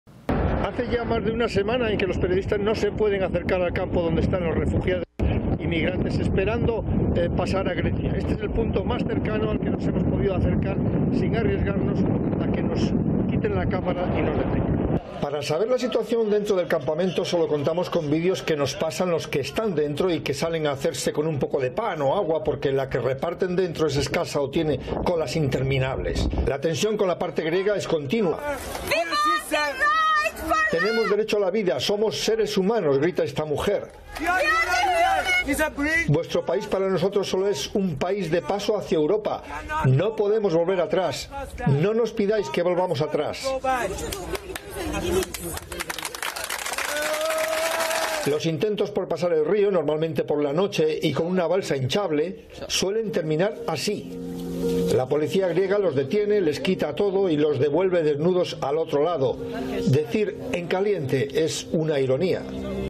Teleprensa33 | Inmigrantes de Siria piden ser aceptados en Europa
Fuerzas Armadas de Turquía desmantelan campamentos improvisados en la zona fronteriza con Grecia donde albergan cientos de inmigrantes, en su mayoría provenientes de Siria que intentan llegar a Europa.

Lunes 9 de marzo de 2020